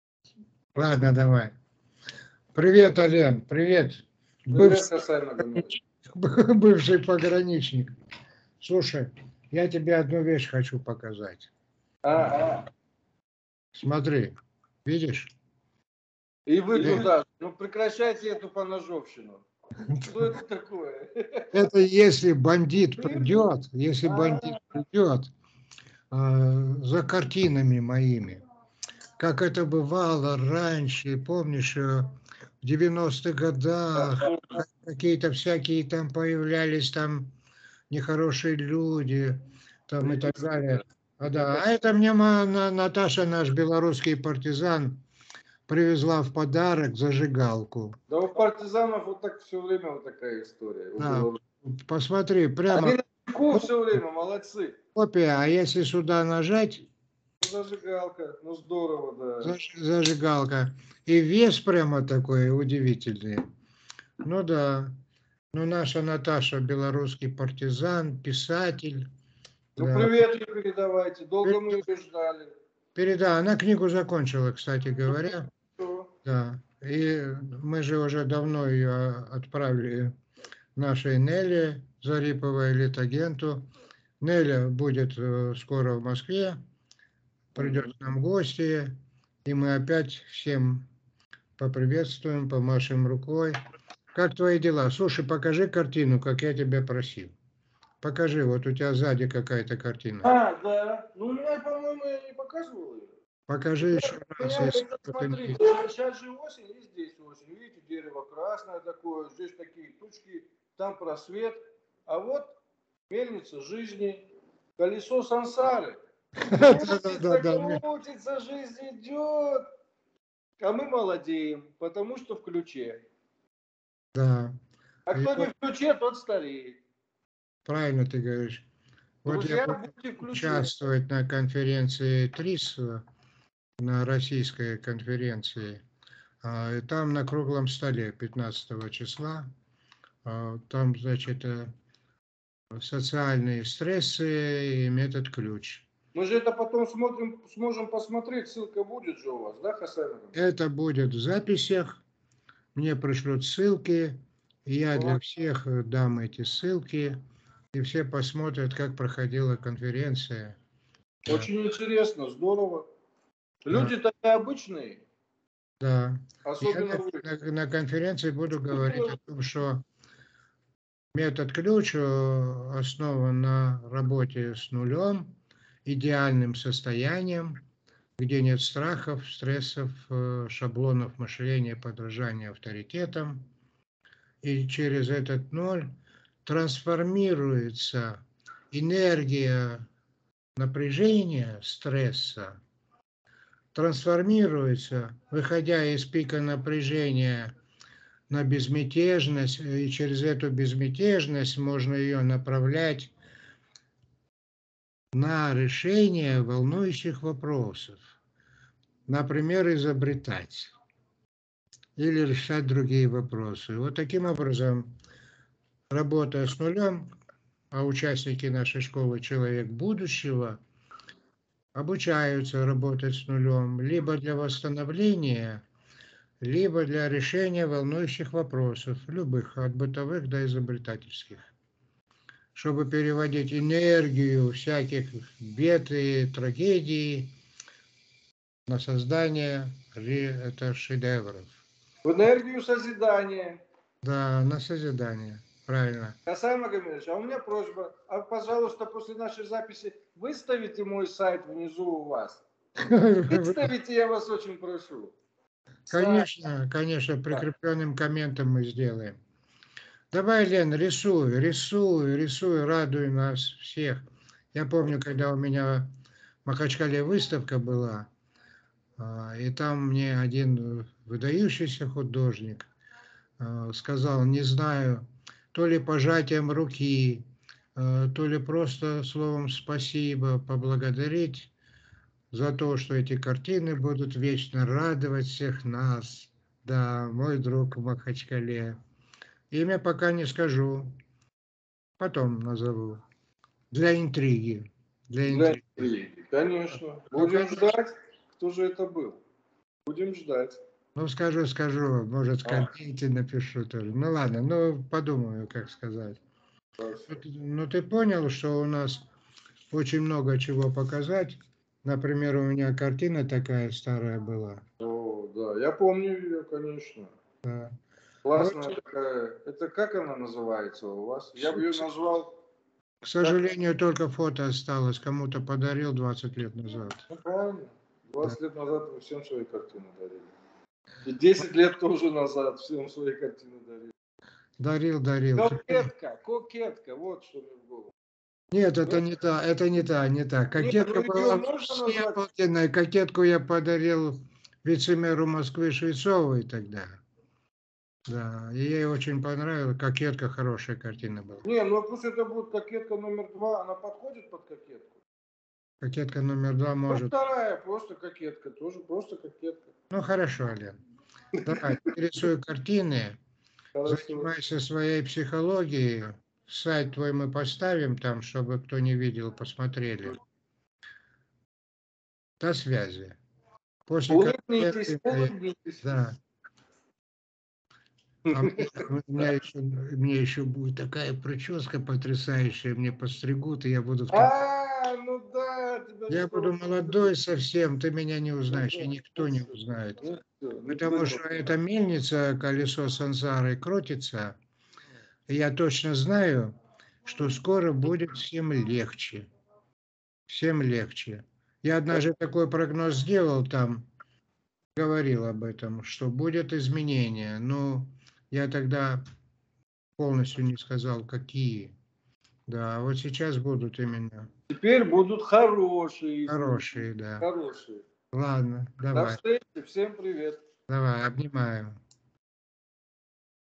Ален и Хасай Алиевы о картинах, мельнице Сансаре, конференции РА ТРИЗ, Нулевом состоянии.Метод Ключ
Беседуют Психолог Ален Алиев и доктор Хасай Алиев о трансформации через Ноль энергии стресса в ответы на волнующие вопросы в быту, искусстве, изобретательстве.